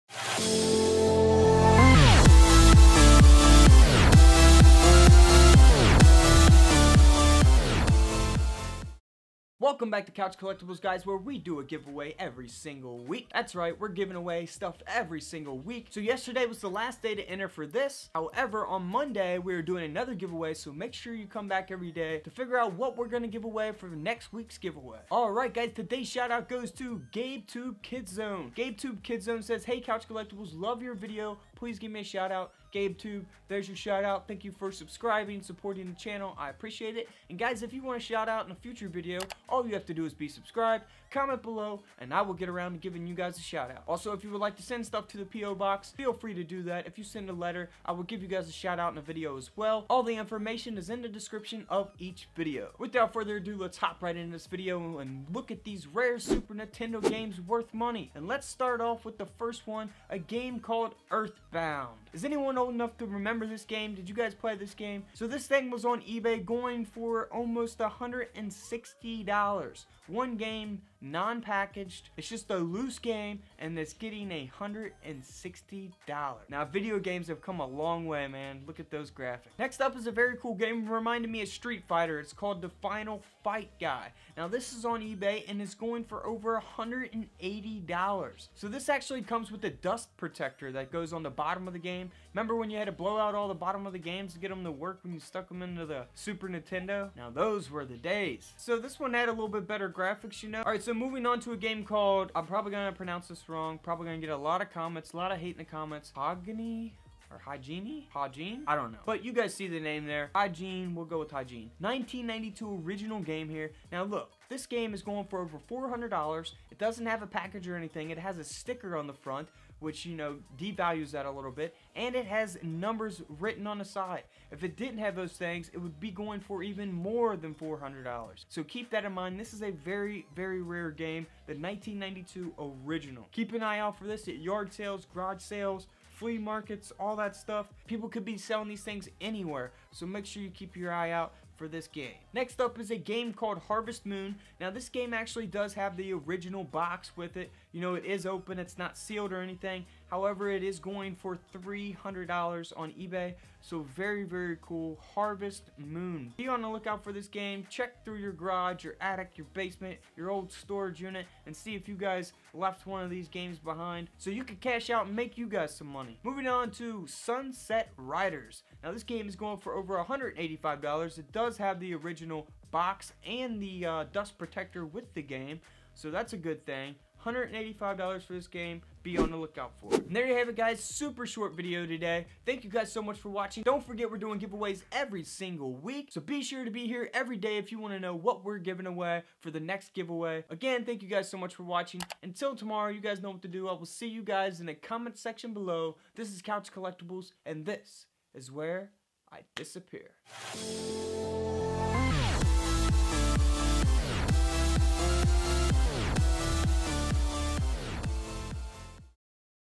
RARE SUPER NINTENDO GAMES WORTH MONEY - VALUABLE SNES GAMES WORTH MONEY!!
These are rare Super Nintendo games worth money. You can easily find rare games at flea markets, yard sales, thrift shops, antique stores or even in your attic or garage! Your game collection could contain these valuable Nintendo games worth money! Be on the look out for the most expensive video games including games that have sold for thousands of dollar on eBay!